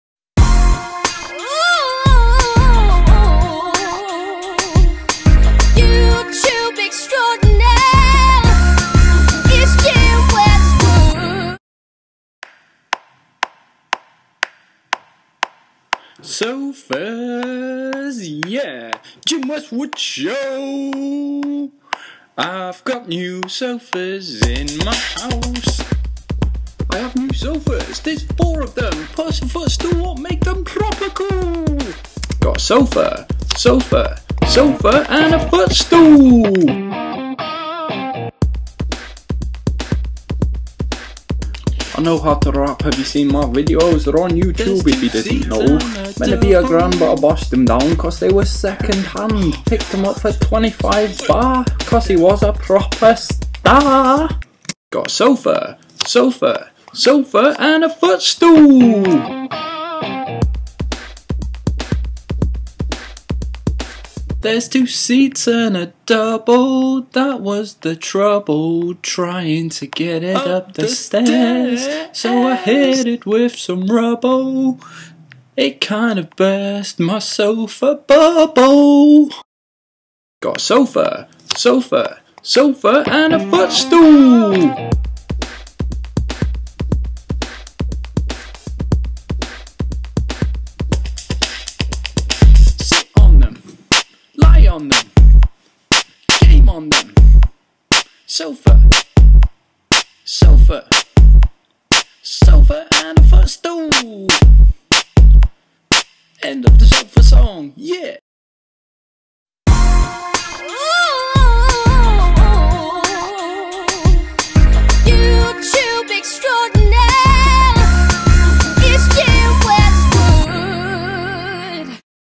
sofa song verry funny epic no1 gymwestwood
this is the first ever gymwestwood music video , about sofas , it has a rap echos , and its all done buy my self with a web cam bannging , possibly the funnest vid i have done. verry funny